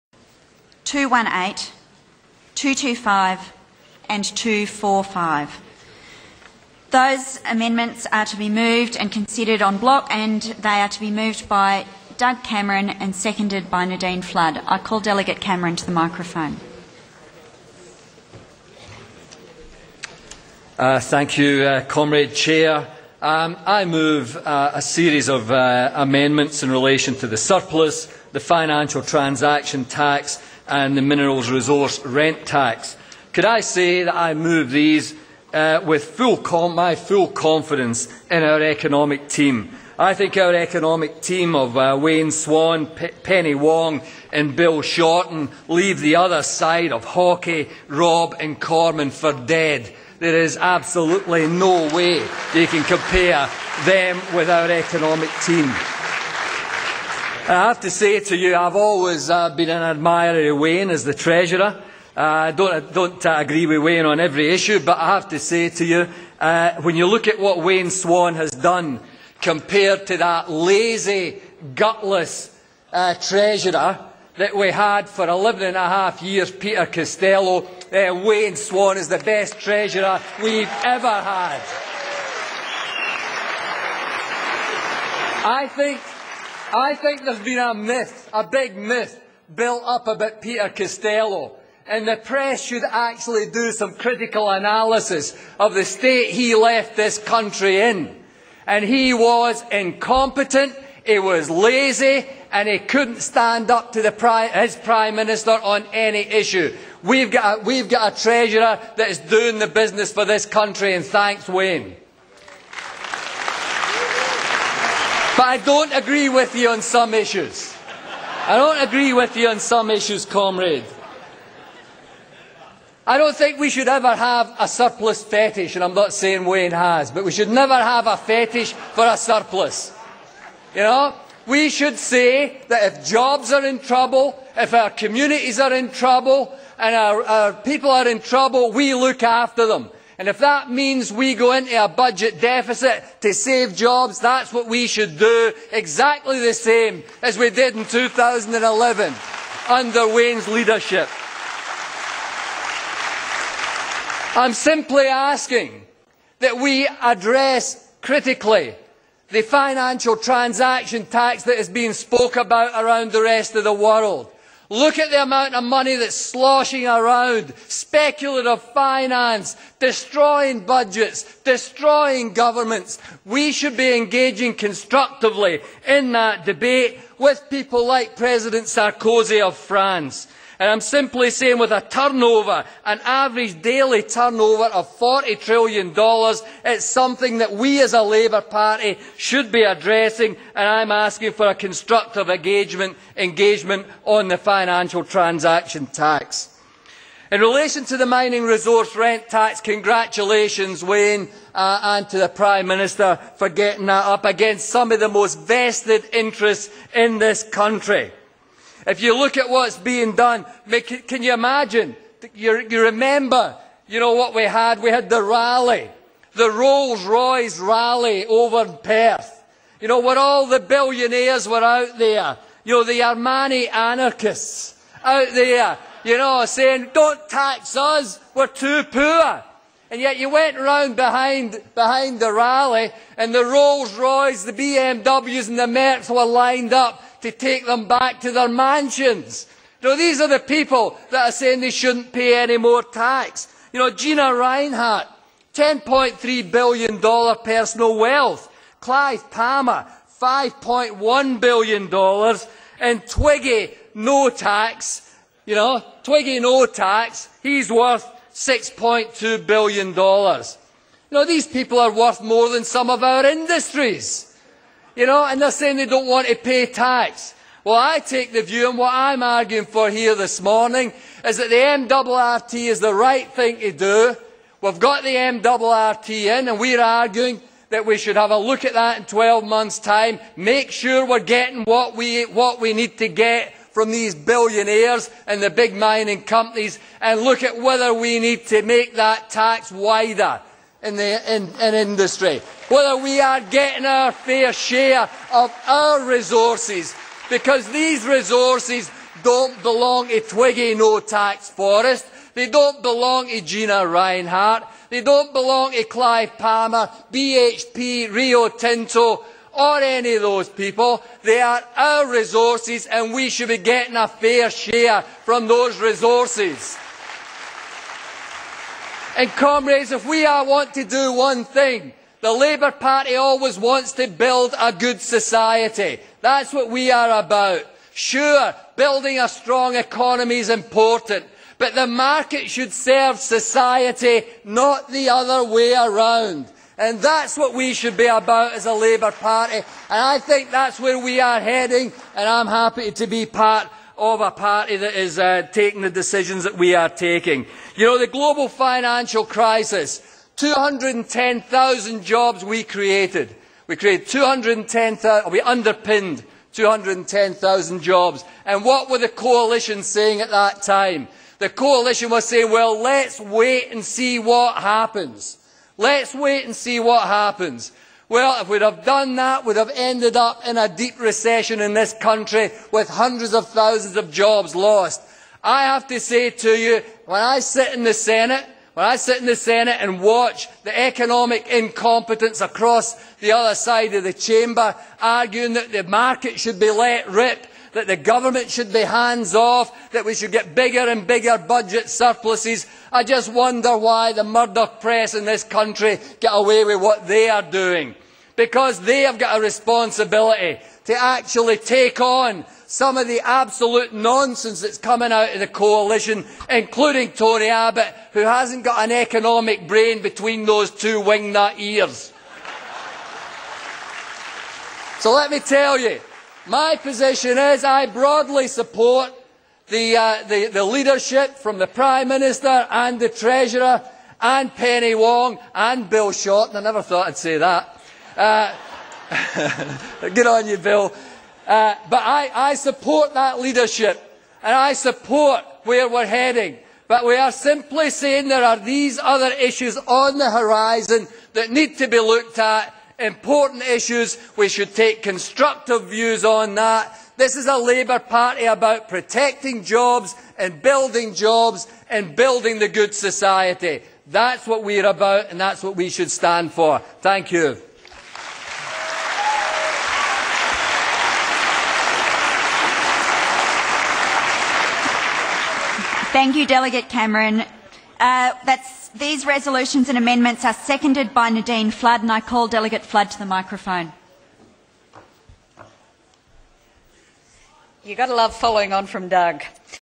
ALP National Conference - Economics - 2nd December 2011
Senator Doug Cameron speaking to the ALP National Conference about economics, surpluses, the Coalition's economic record and Peter Costello's time as treasurer.